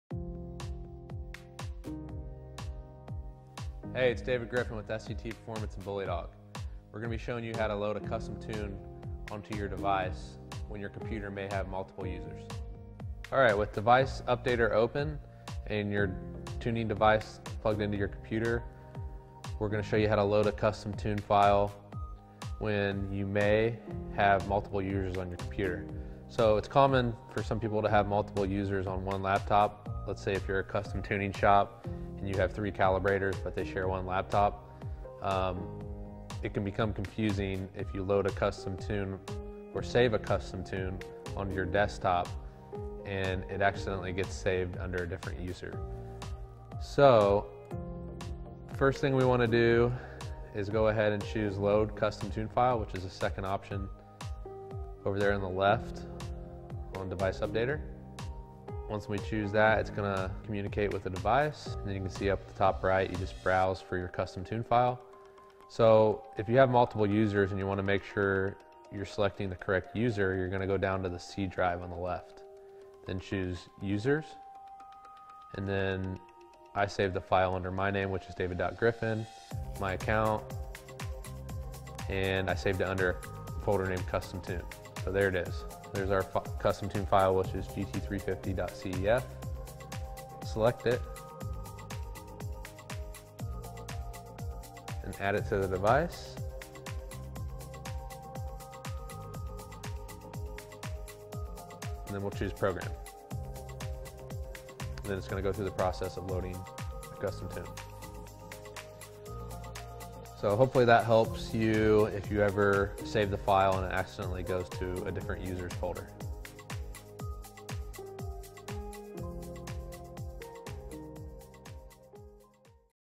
How To Load a Custom Tune when Your Computer May have Multiple Users
Ready to learn how to load a custom tune when your computer has multiple users?  This video shows you how!  Happy Tuning!